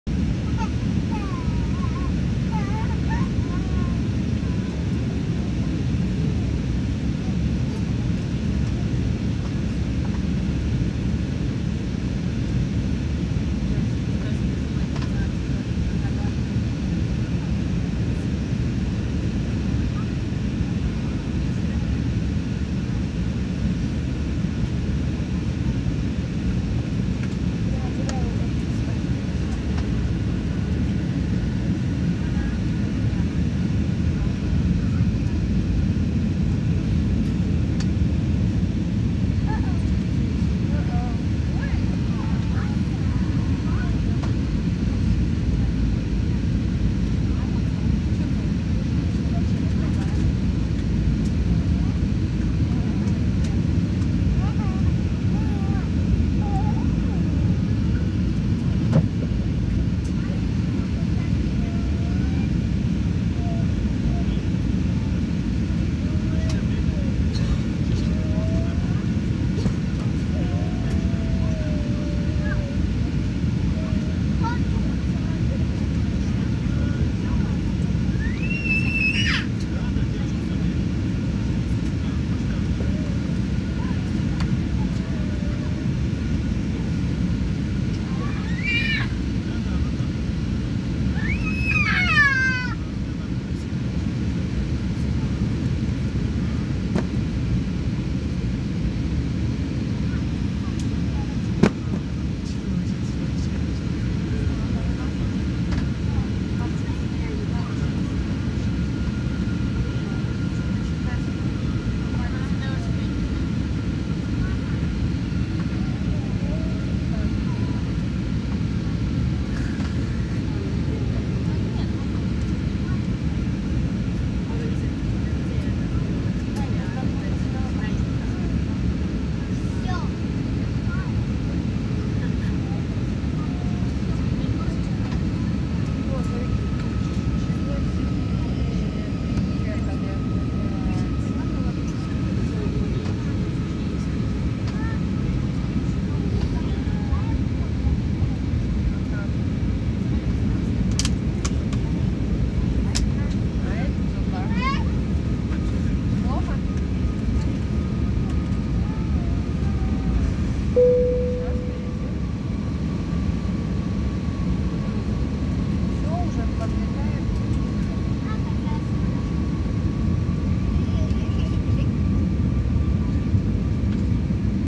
Aircraft has flown through clouds and frighten.
The aircraft flew through clouds and got vibrate wings and it was bit scary but a new experience.